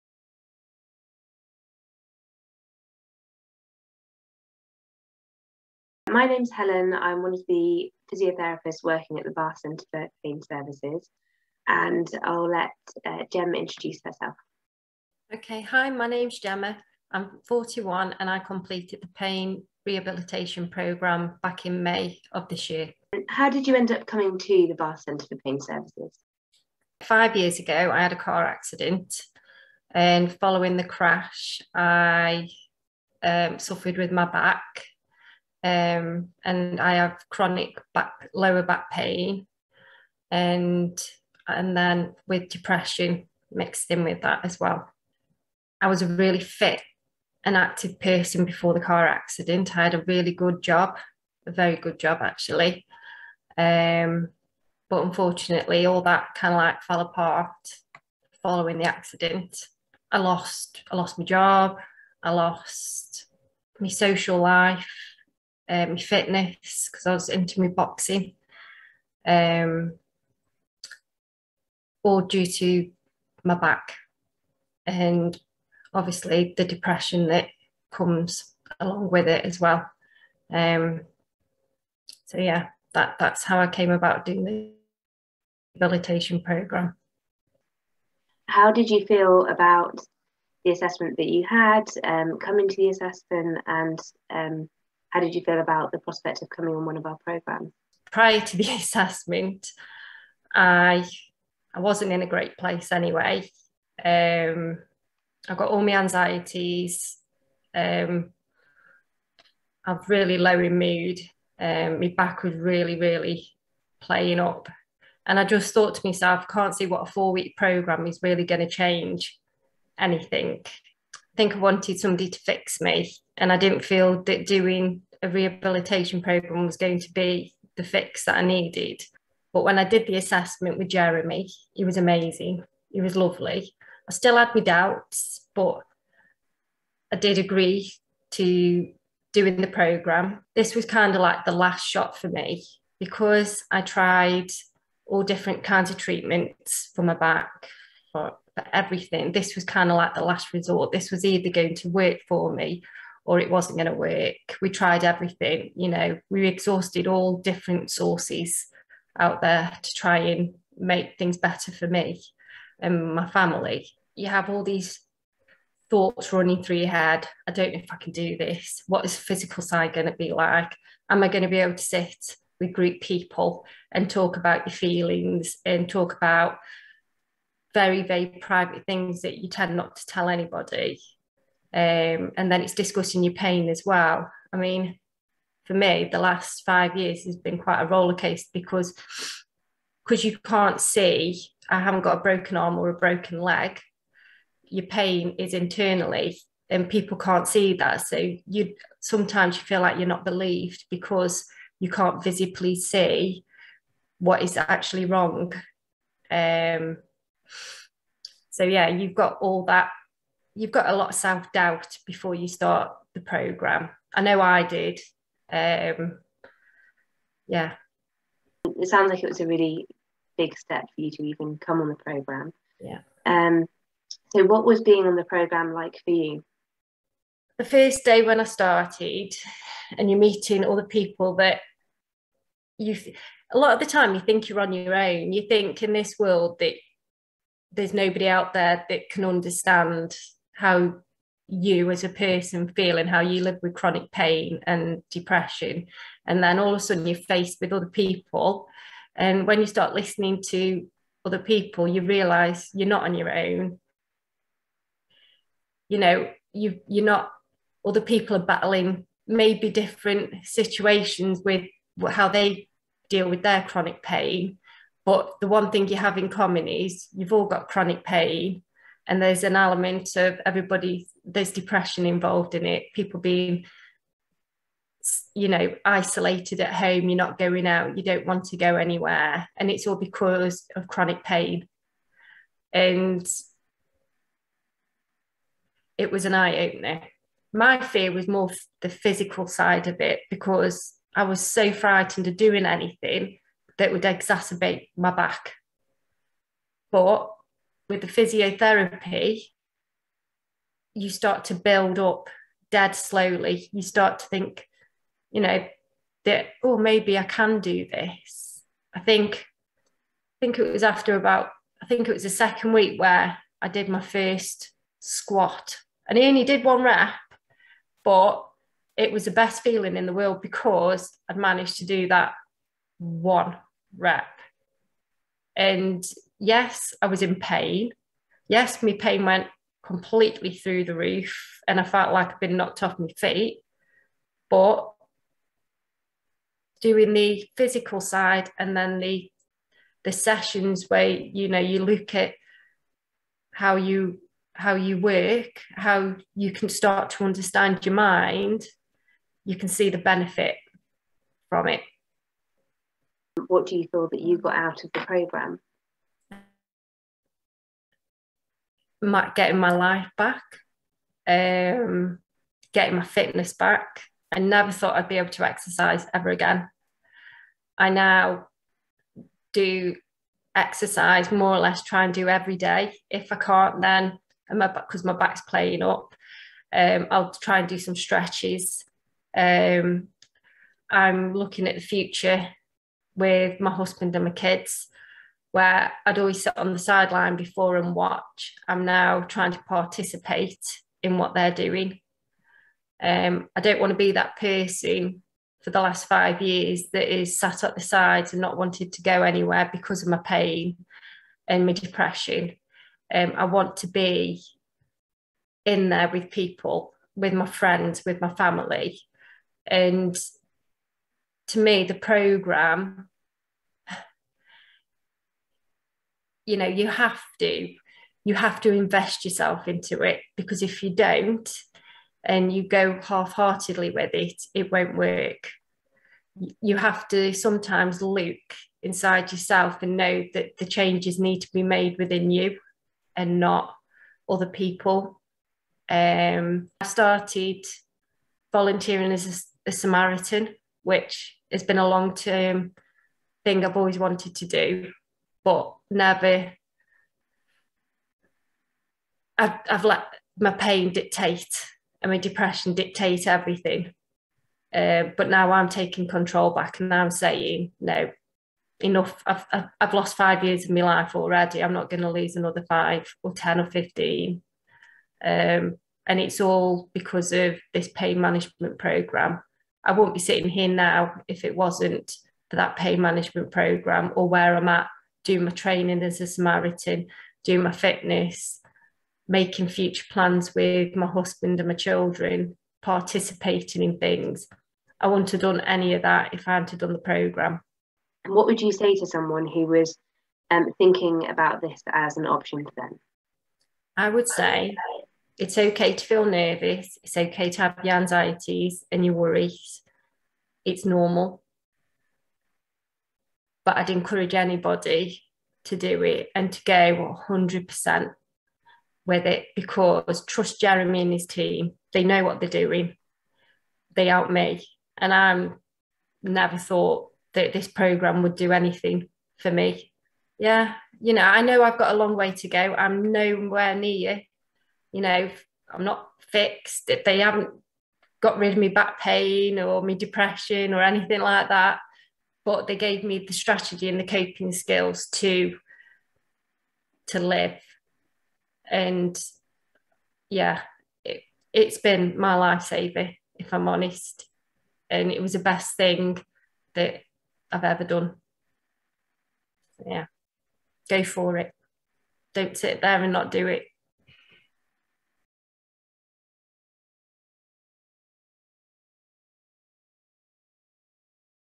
Patient interview following a four week Pain Management Programme
Timecodes
0:00 Intro
0:23 How did you end up coming to the Bath Centre for Pain Services?
1:28 How did you feel about the assessment and coming on one of our Programmes?
3:44 What was being on the Programme like?
5:53 What do you feel you got out of the Programme?
8:08 What would you say to someone thinking about this as an option for them?